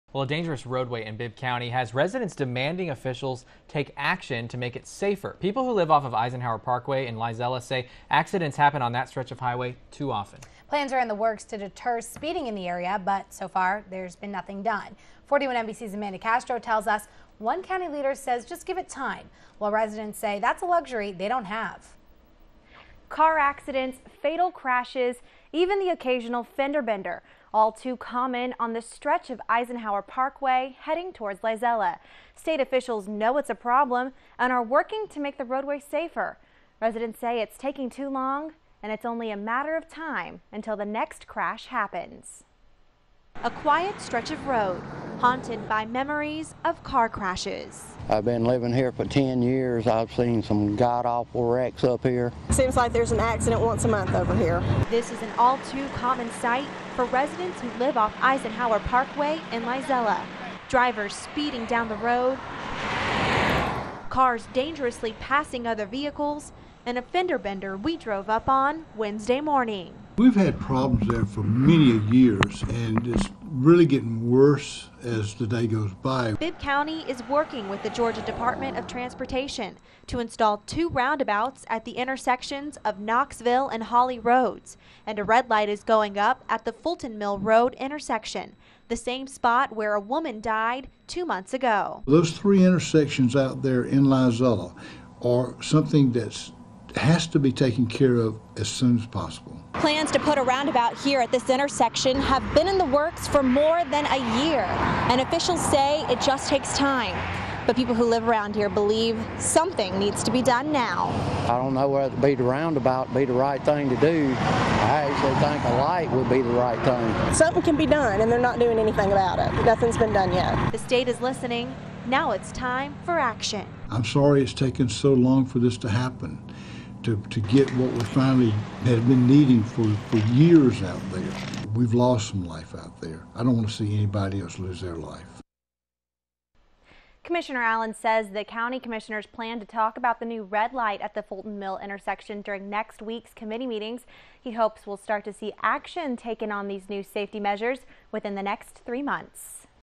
41NBC/WMGT- Driving Dangers- 5.1.13
A dangerous roadway in Bibb County has residents demanding  officials take action to make it safer.